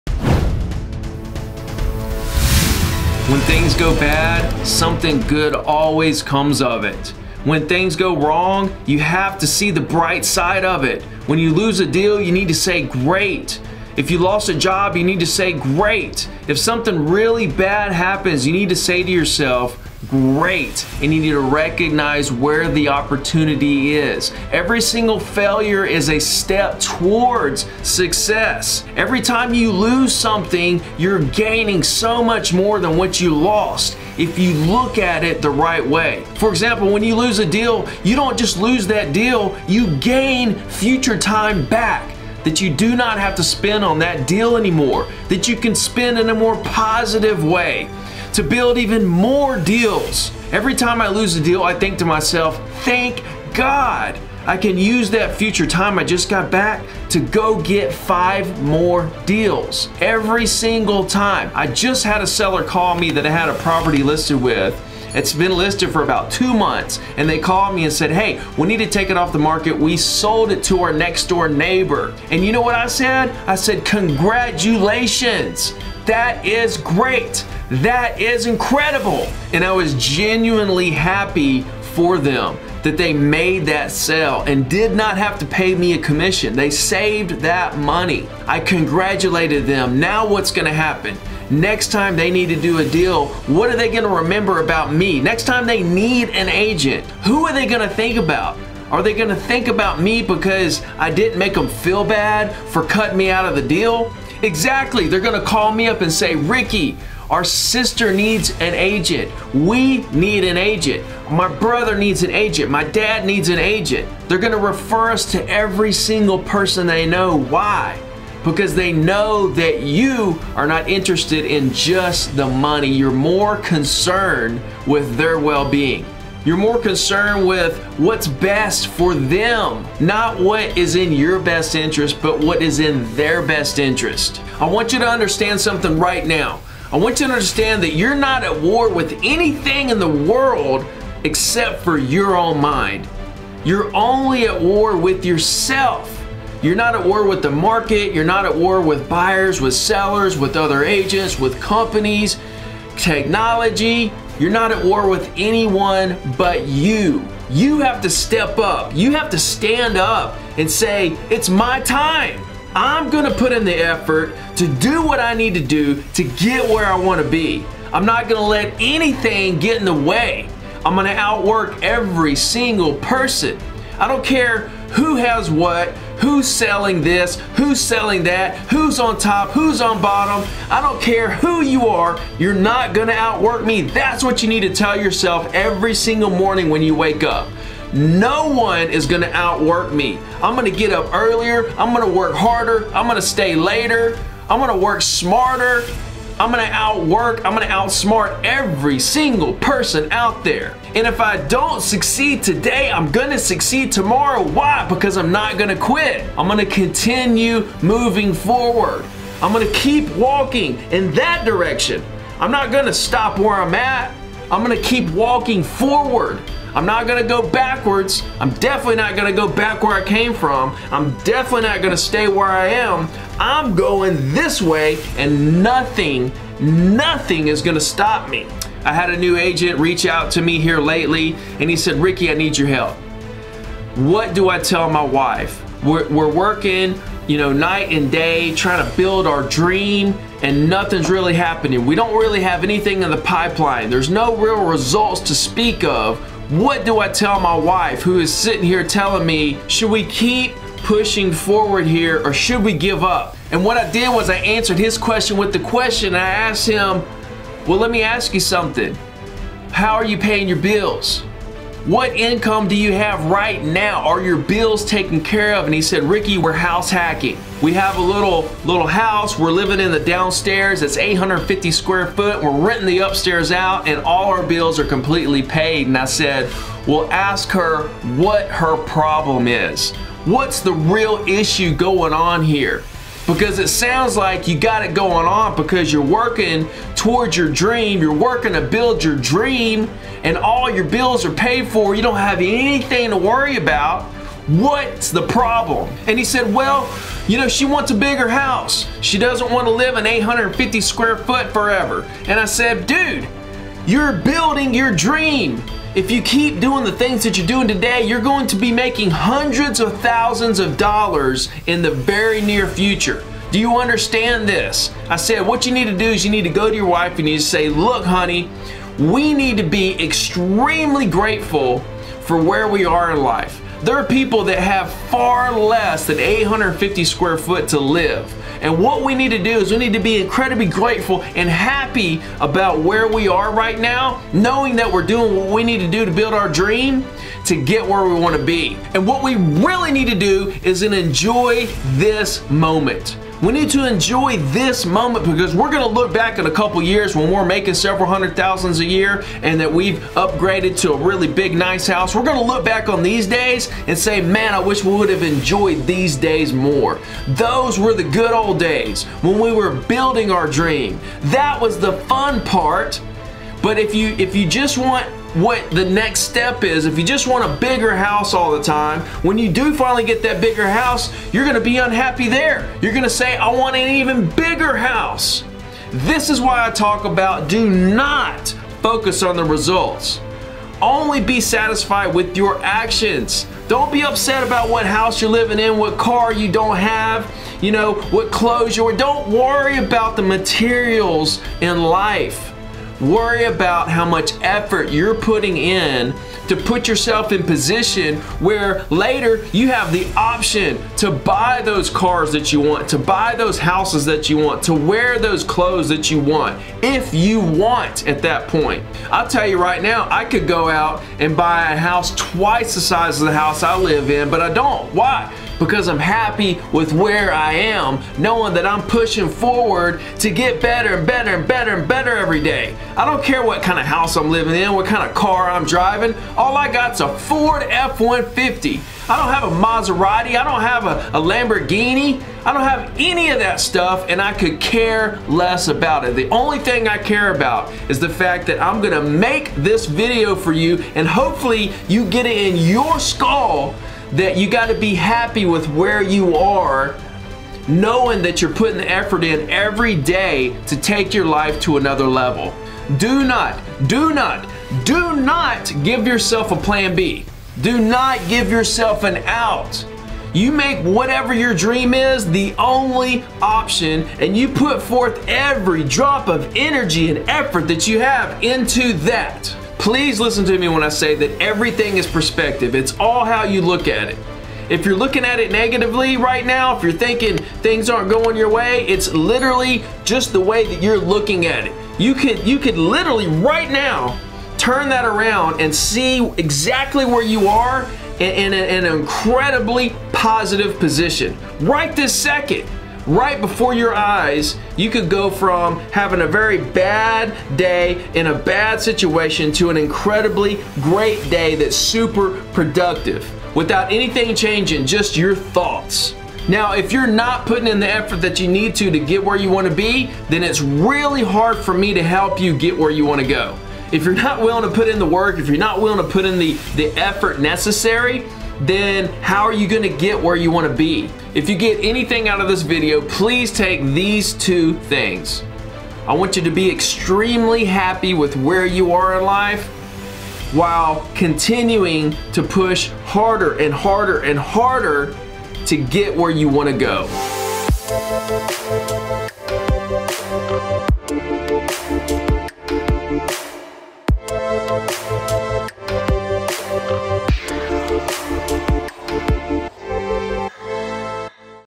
How to Overcome Failure and Keep Pushing
Download all my free real estate scripts and the 60 Day Challenge for real estate agents in the free courses Once you realize that for you to overcome failure you must embrace the failures, everything changes. Personally, I invite failure and try to go so big that failure is the most likely scenario. I way I overcome failure is by enjoying failure as a necessary part of the process. Without failure, there is no success.